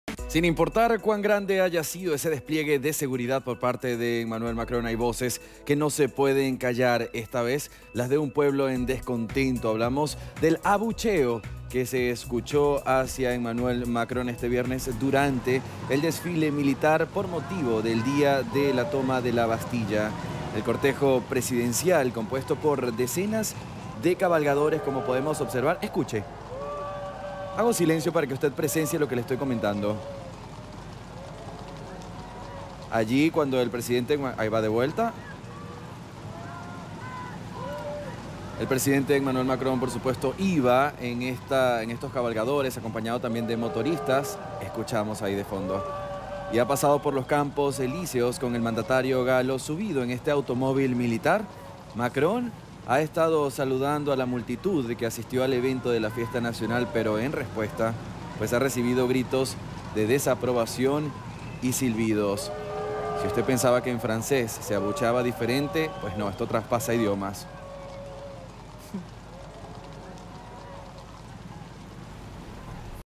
FRANCIA | Una multitud abuchea a Macron durante un desfile militar en París
👉 Una multitud ha abucheado al presidente de Francia, Emmanuel Macron, este viernes durante el desfile militar por motivo del Día de la Toma de la Bastilla.